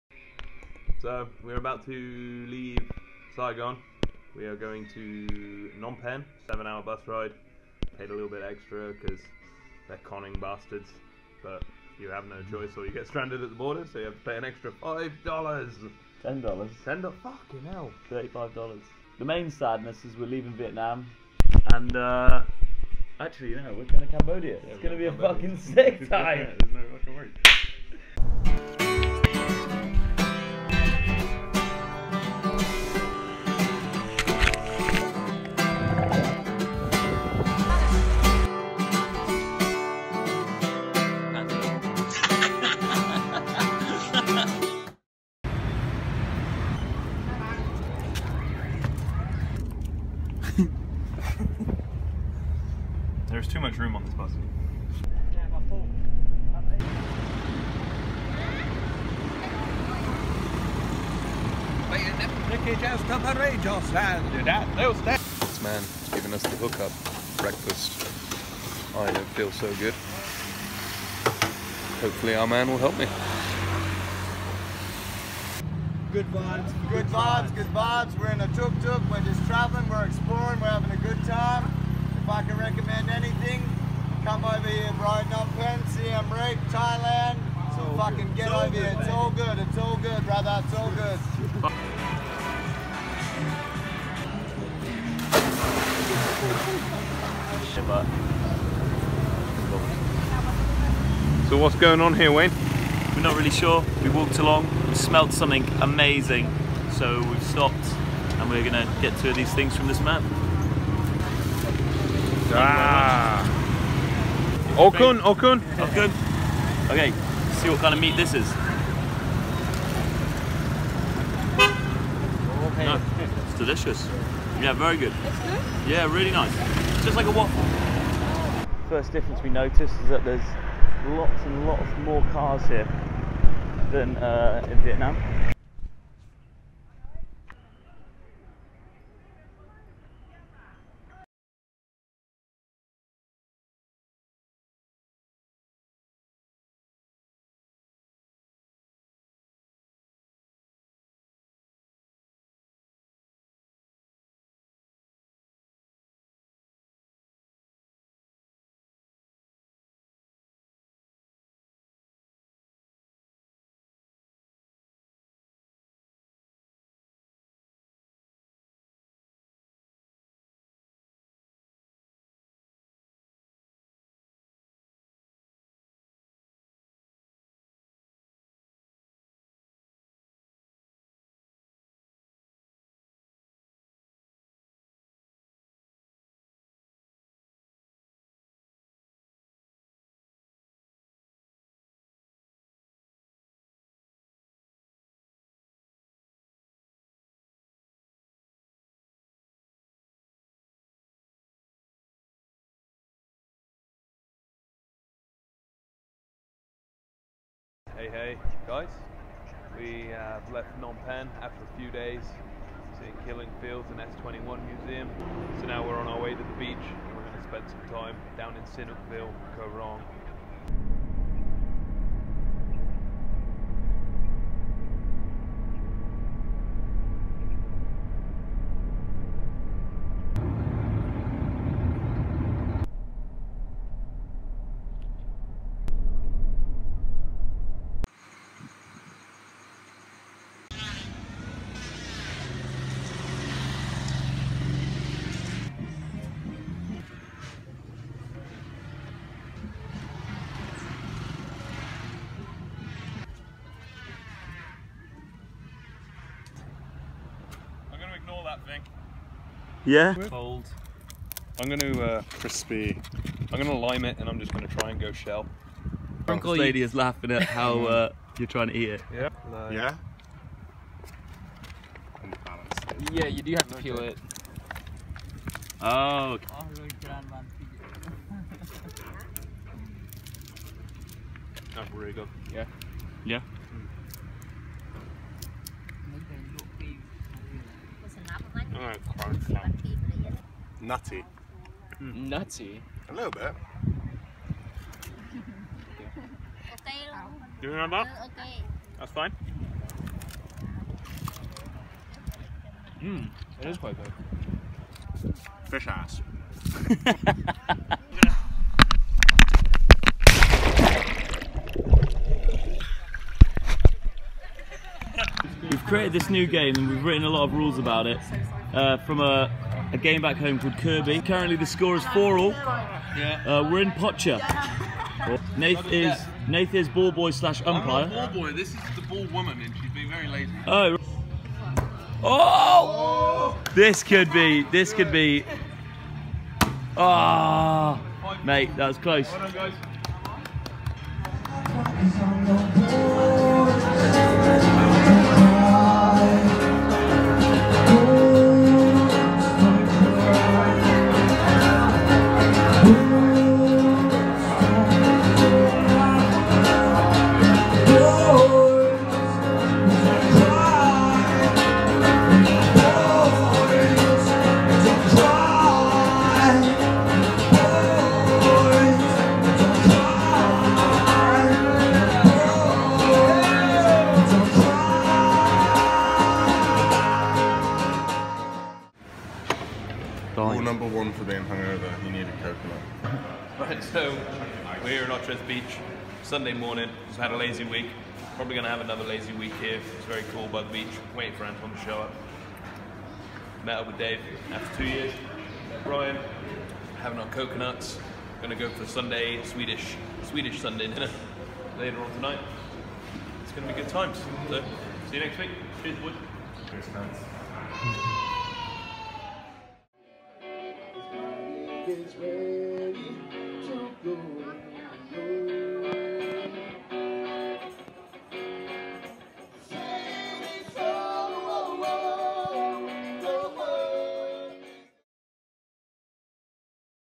#8 Room for activities
The first week in Cambodia. Initially Wayne didn't like the change but he quickly has learned to love it now we're out of the city. Below are the links that are useful from the video. Worth reading up on. now for another week of beach chills